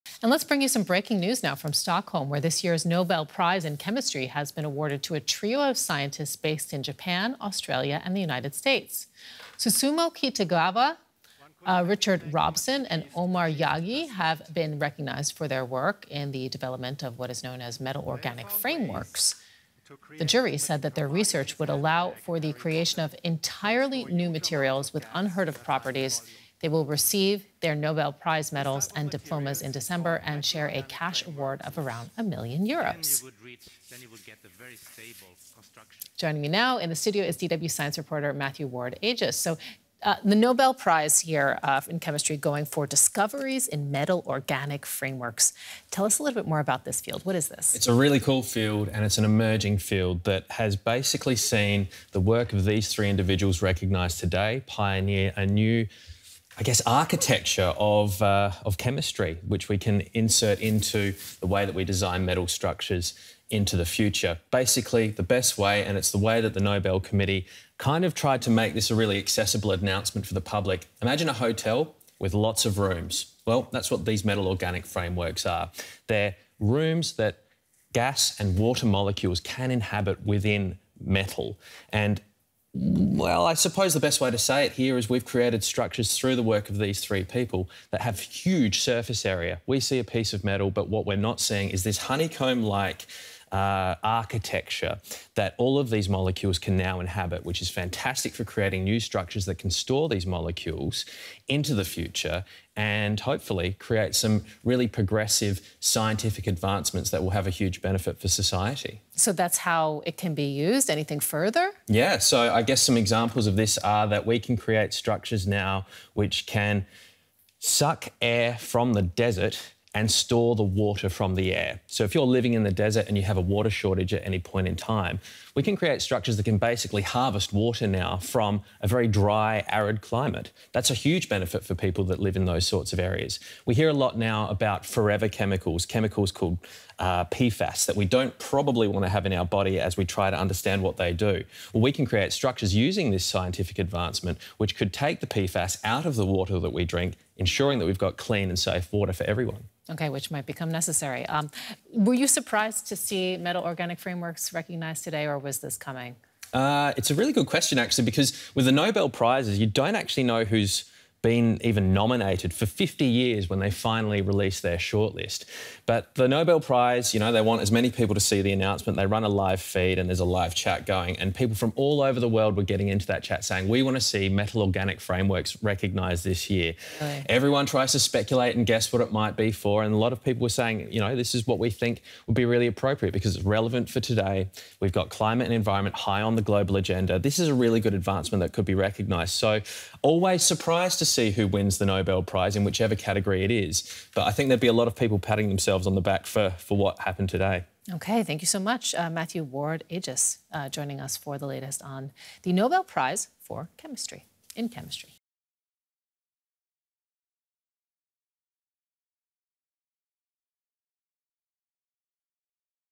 Nobel Prize in Chemistry awarded "for the development of metal–organic frameworks" | DW News
The Royal Swedish Academy of Sciences has decided to award the 2025 Nobel Prize in chemistry to Susumu Kitagawa, Richard Robson and Omar M. Yaghi "for the development of metal–organic frameworks." The new type of molecular architecture developed by the group allows for "large cavities in which molecules can flow in and out. Researchers have used them to harvest water from desert air, extract pollutants from water, capture carbon dioxide and store hydrogen," according to the award-giving body. Their research could provide solutions for combatting climate change.

"A small amount of such material can be almost like Hermione's handbag in Harry Potter. It can store huge amounts of gas in a tiny volume," Heiner Linke, chairman of the Nobel Committee for Chemistry, said during a press conference.